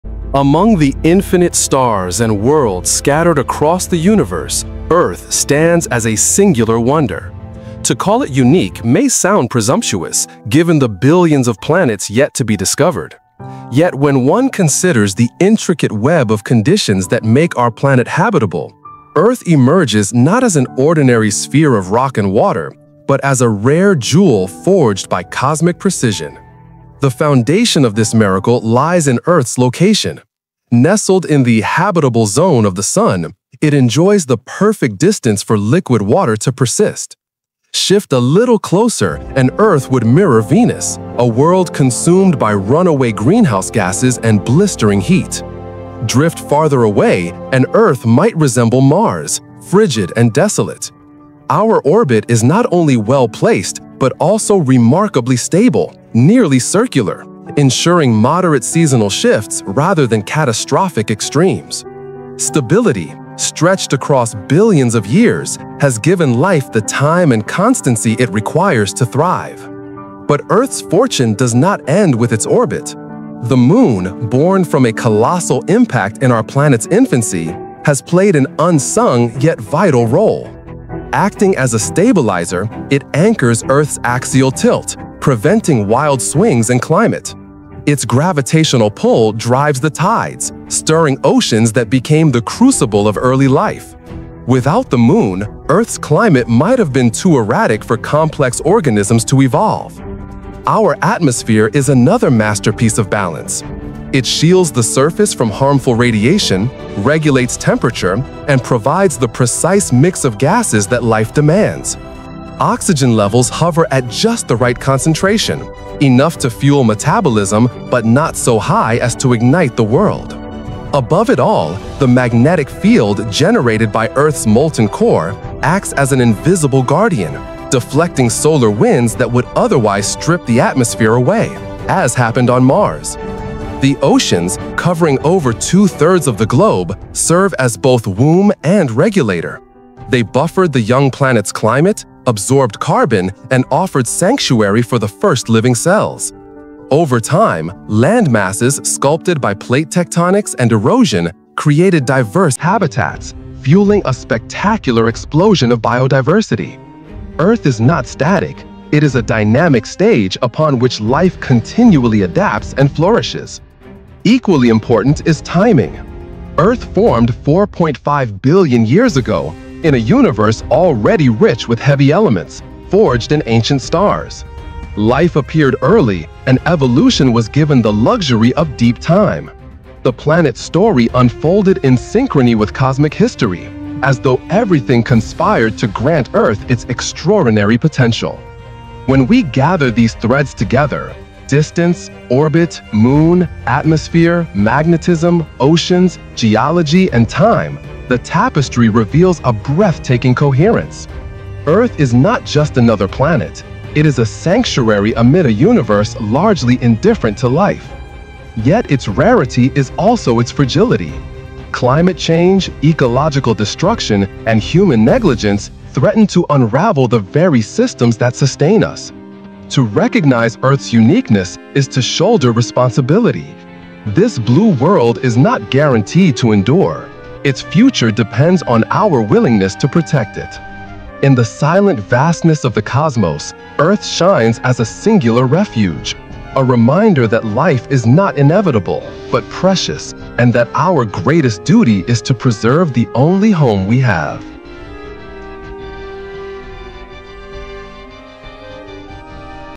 Earth: A Planet Designed for Life?
Earth – the only planet in the Solar System known to sustain life. Its “perfect” position in the habitable zone, stable orbit, protective atmosphere, magnetic field, and vast oceans have created the ideal environment for evolution. This video explores why Earth seems almost “designed” for life, and what makes it truly unique in the cosmos.